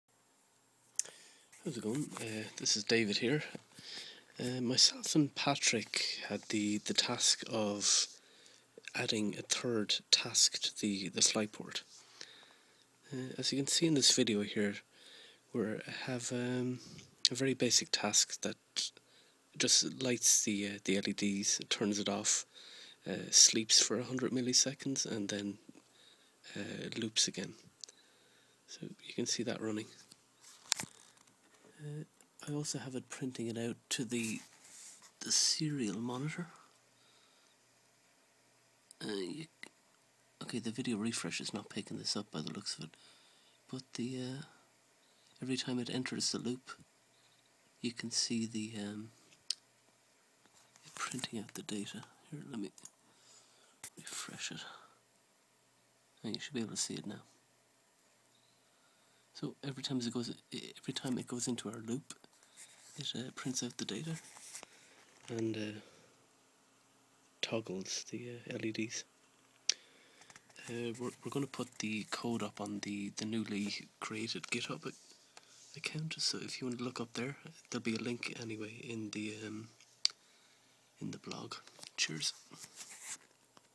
Adding a new task to FreeRTOS
Hi folks, David and Patrick here, we had the job of investigating the means to add a third task to FreeRTOS which powers our OpenPicus Flyport embedded system.

With this complete we can now begin to implement the functionality required to power the AirfishBowl system!